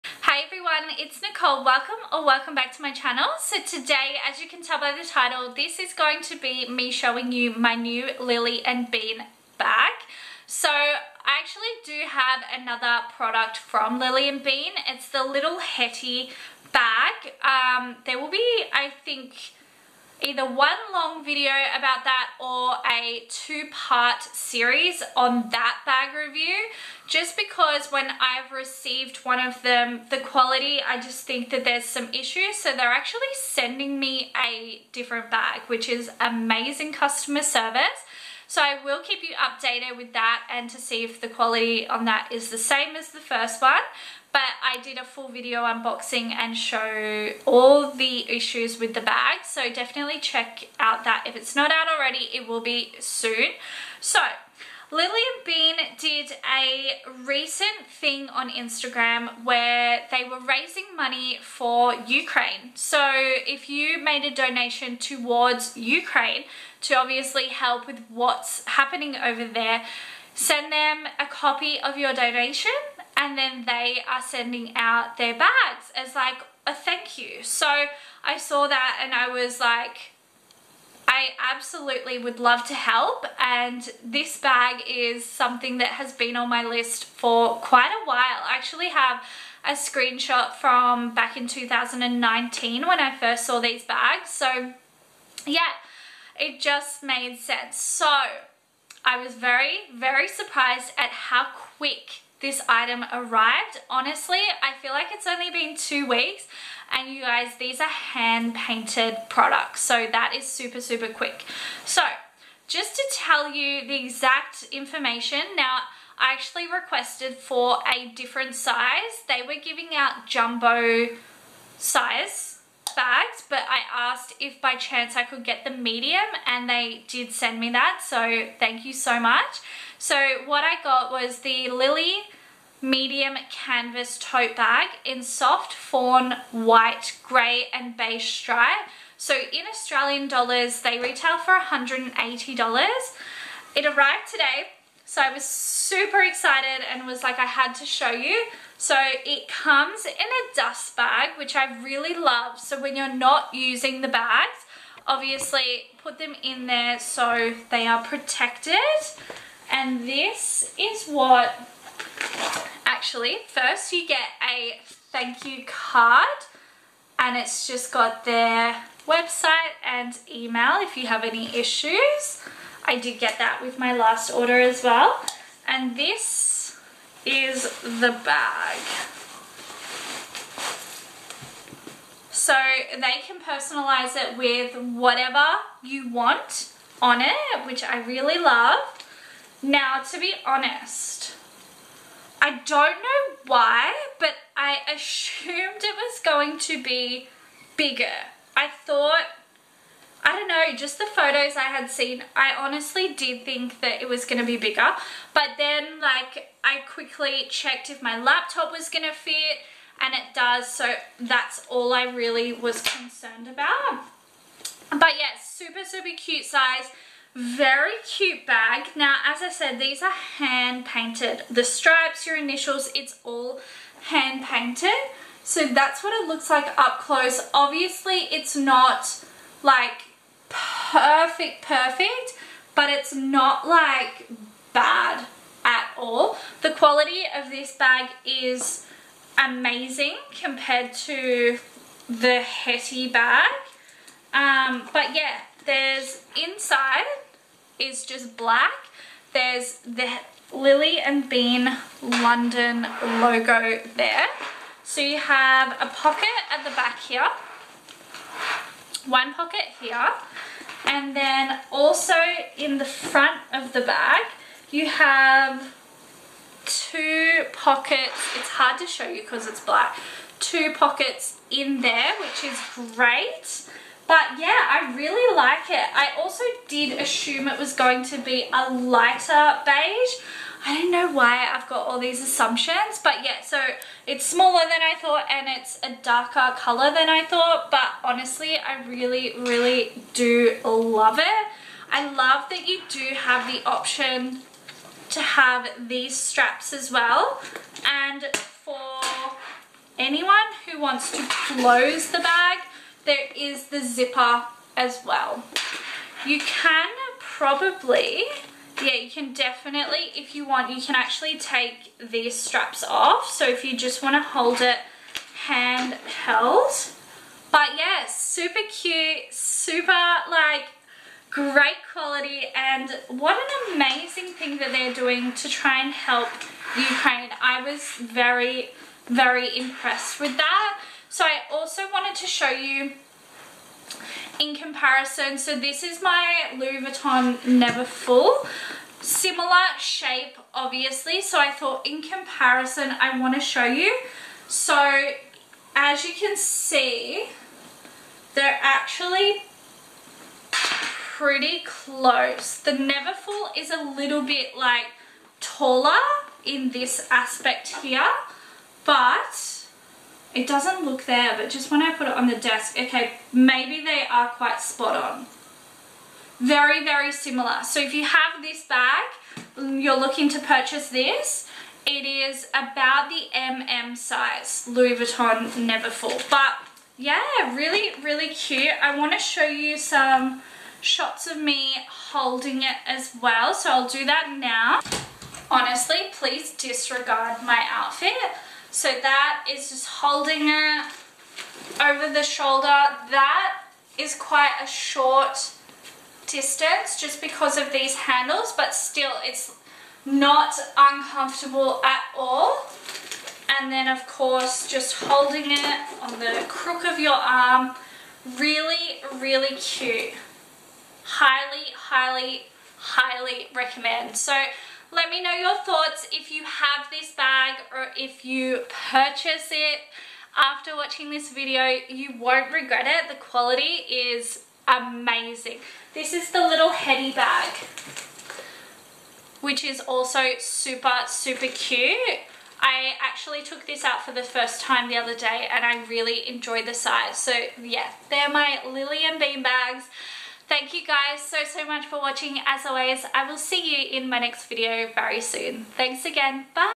Lily & bean personalised tote unboxing & honest review!! & First impressions!
Hi babes!

Products Used -
Lily & bean personalised tote unboxing